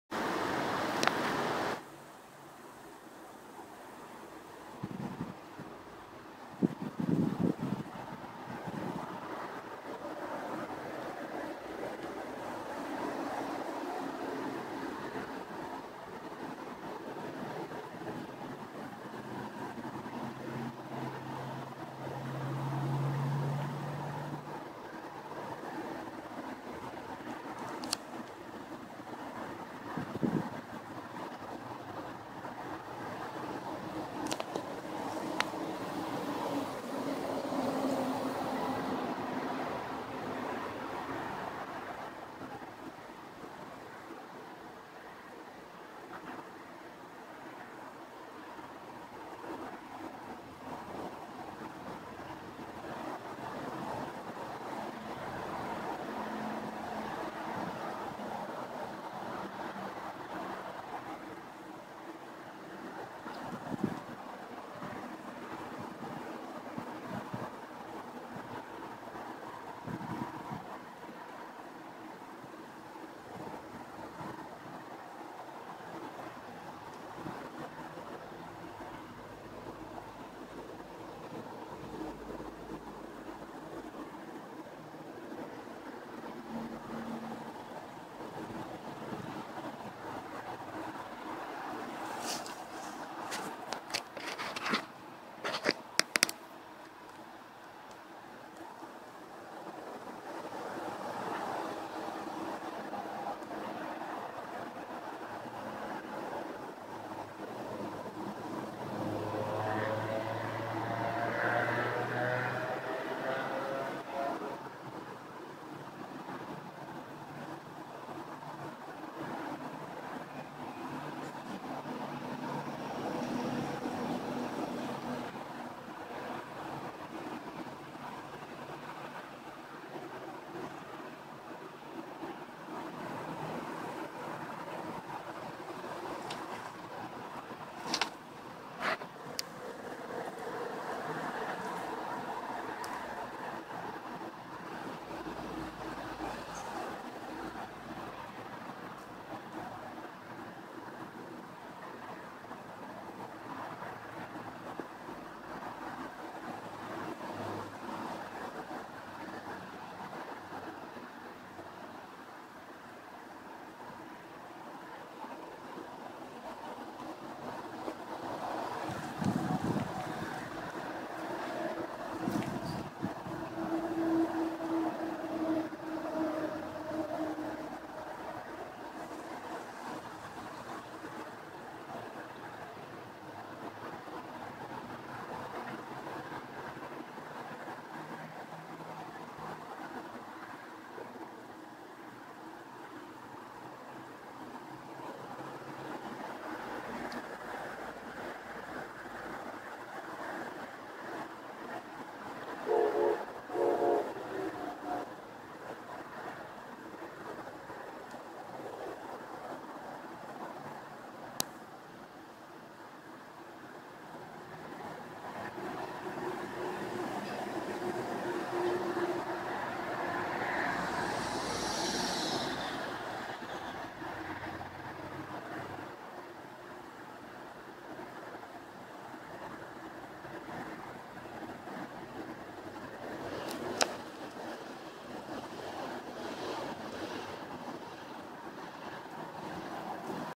Moth laying eggs 21 08 2013
papillon de nuit en train de pondre...